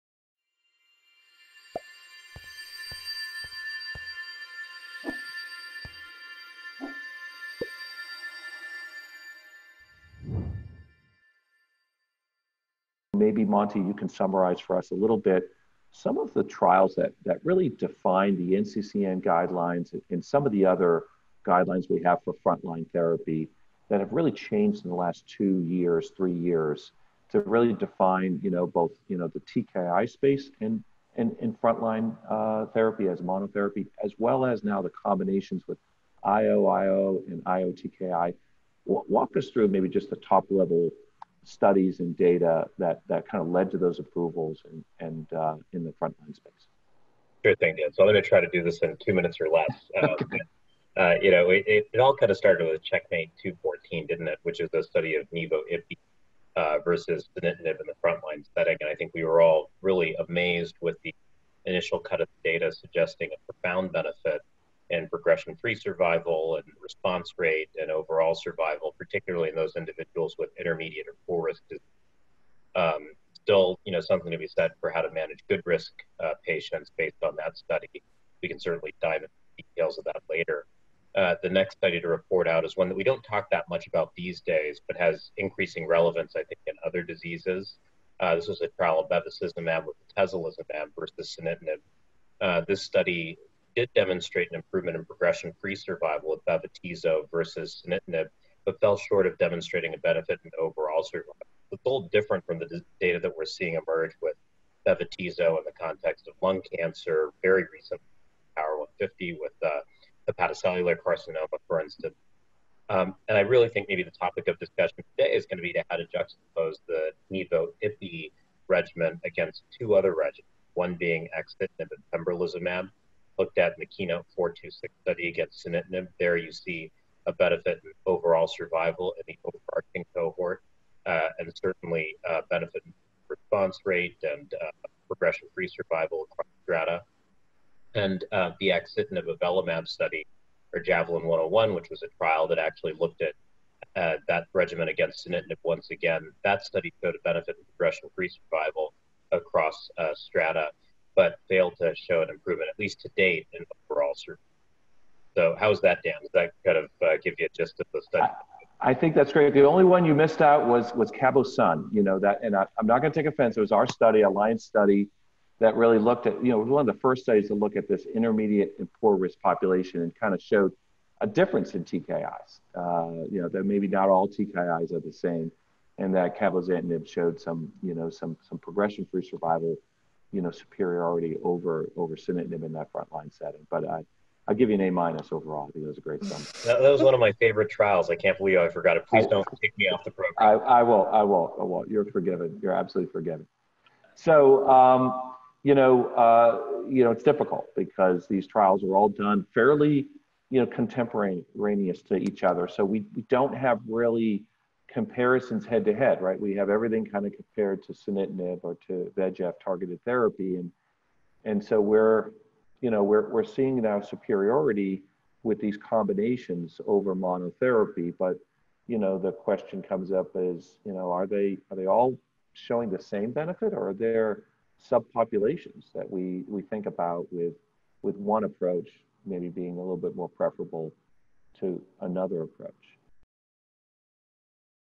Clinical Trials Defining the NCCN Guidelines for Frontline Treatment of mRCC
Experts discuss the NCCN guidelines and how they impact frontline treatment of mRCC.